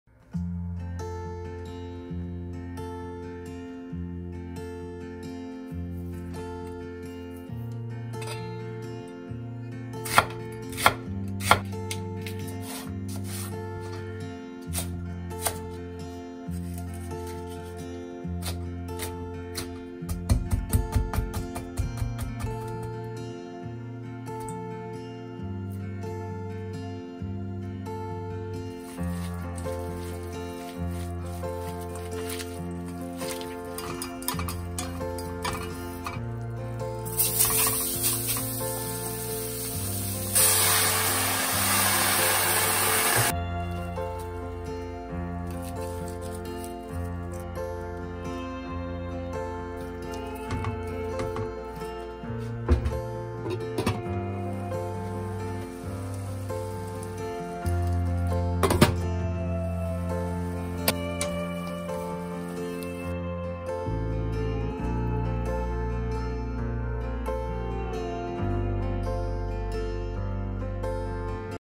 깊은 맛을 제대로 살린 어요남표 시래기 된장볶음
된장의 깊은 맛을 제대로 살려 지져낸 시래기볶음,,,씹을 것도 없이 샤르르 넘어 간답니다~